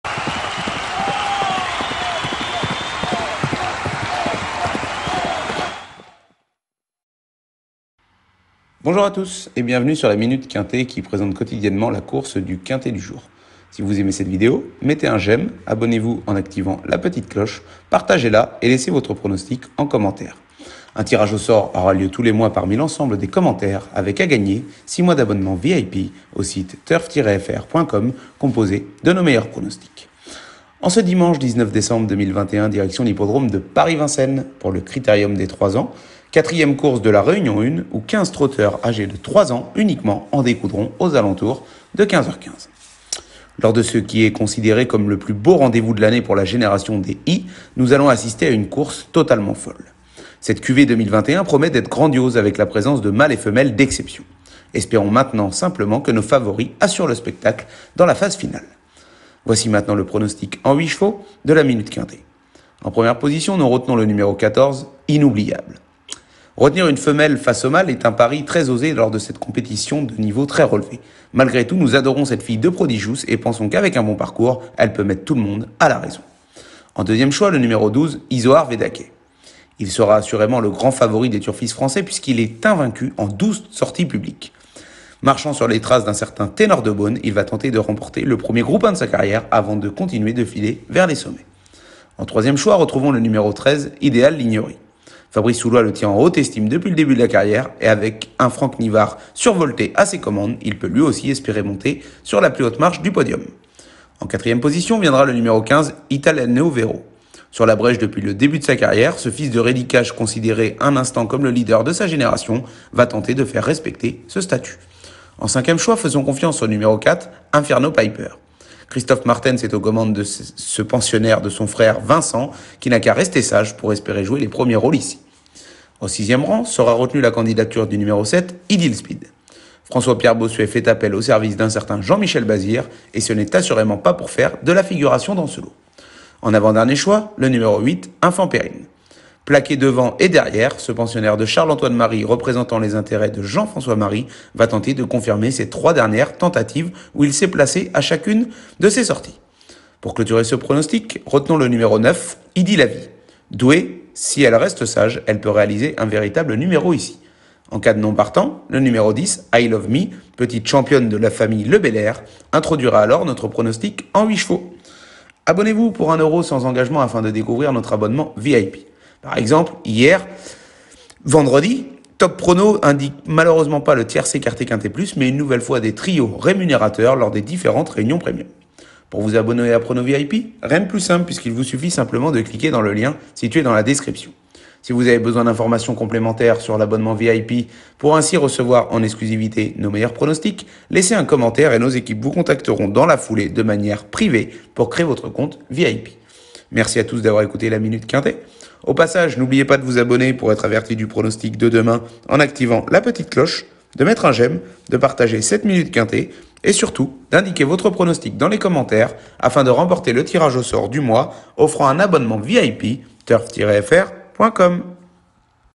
Minute Quinté TURF : CRITERIUM DES 3 ANS - Dimanche 19 Décembre 2021 - Paris Vincennes 🏇 PMU #263105
🥇 Retrouvez le pronostic exclusif de notre expert Turf Lucius Simenon pour le CRITERIUM DES 3 ANS - Dimanche 19 Décembre 2021 à l'hippodrome de Paris Vincennes en France.

✅ NOS LOGICIELS TURF :
✔️ Logiciel TURF INFO Professionnel Edition: